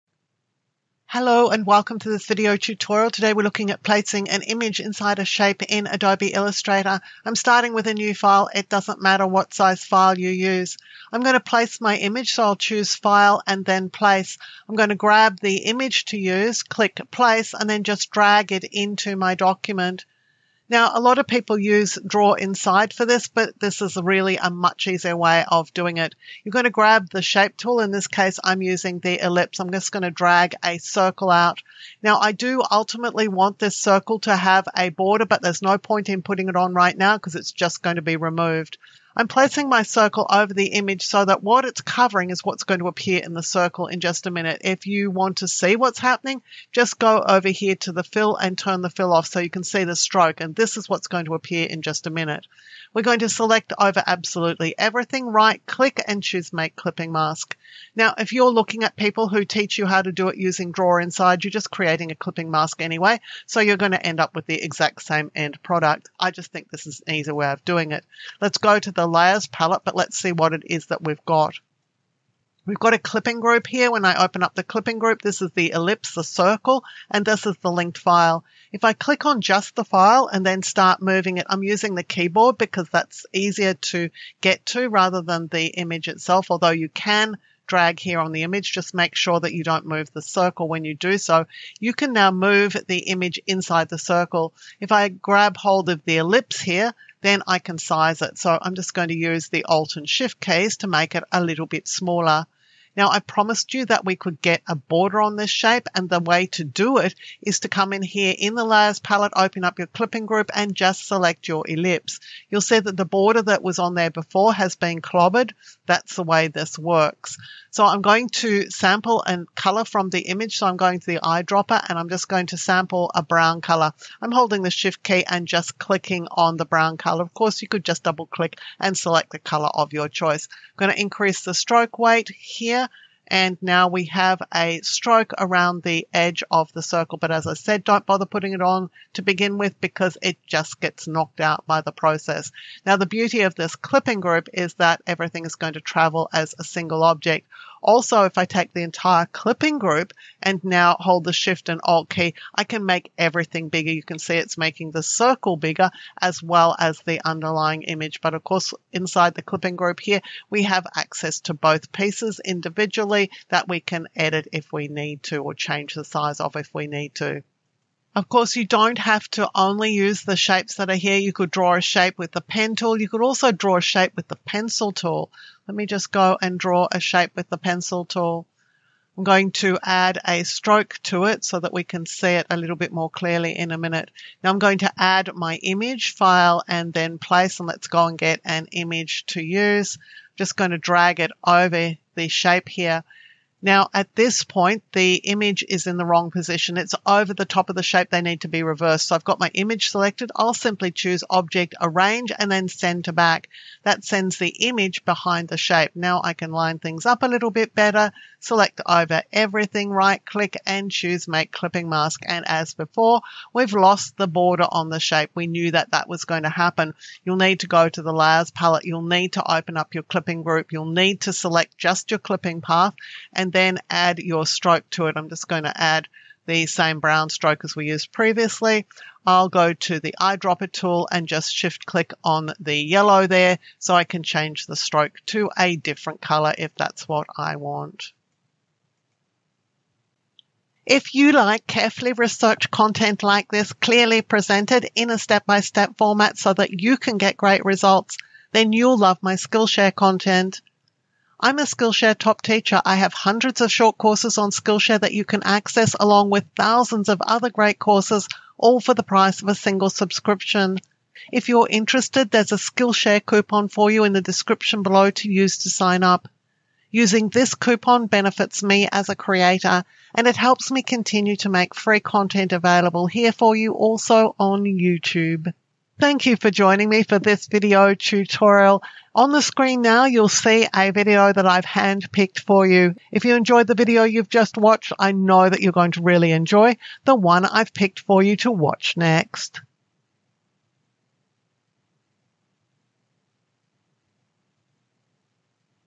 Illustrator - Image in Shape - Super Quick & Editable - Step by Step
Illustrator - Image in Shape - Super Quick & Editable - Step by Step - Fully Narrated
Learn to add an image to a shape in Illustrator. I'll show you a foolproof way to do this and explain how to edit the final result and how to add a border to the image. This video is fully narrated and shown step by step.

Illustrator - Wrap a Doodle Design Around a Circle - Turn a Straight Path into a Circular One